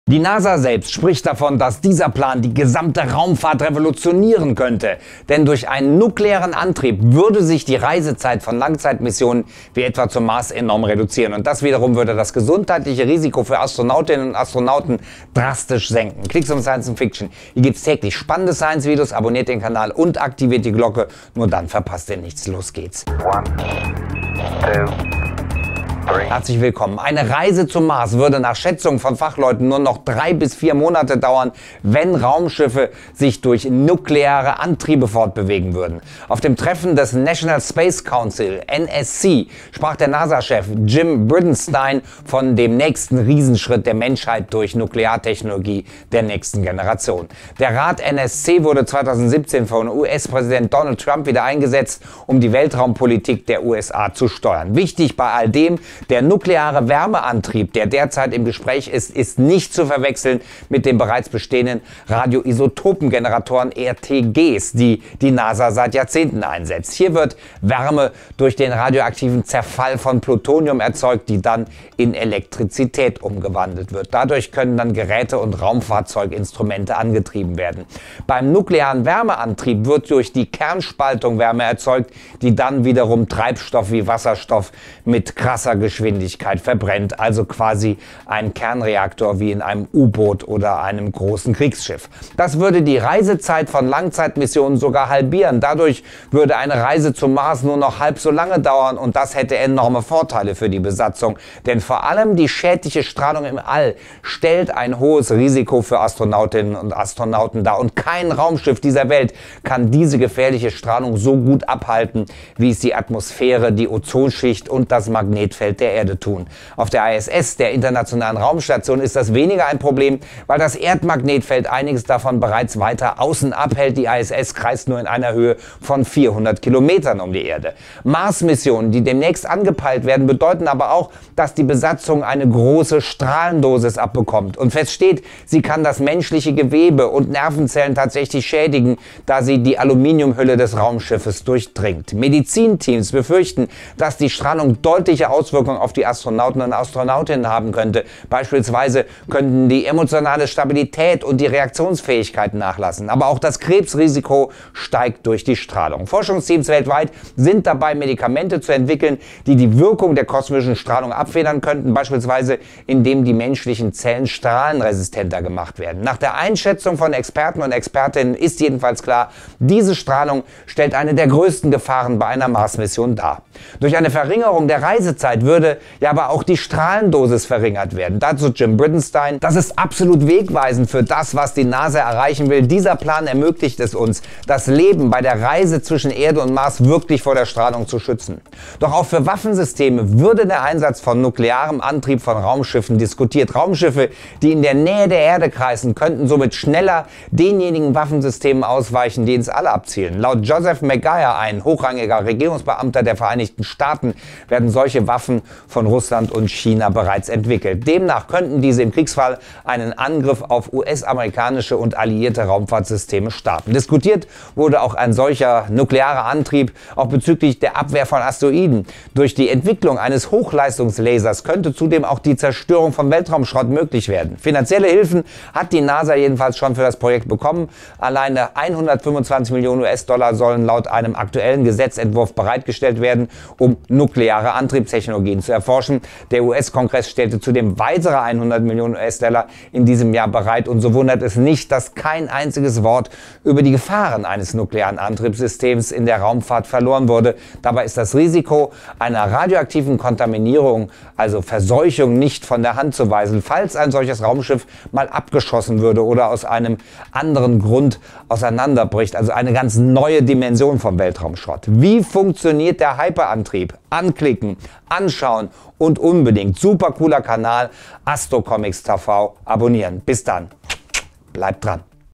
Reise zum Mars: Revolutioniert nuklearer Antrieb die Raumfahrt?!
Die NASA selbst spricht davon, dass dieser Plan die gesamte Raumfahrt revolutionieren könnte: Denn durch einen nuklearen Antrieb würde sich die Reisezeit von Langzeitmissionen wie etwa zum Mars enorm reduzieren. Und das widerum würde das gesundheitliche Risiko für Astronautinnen und Astronauten drastisch senken!

Skript: Christine Kirchhoff
Schnitt: Julian Zahn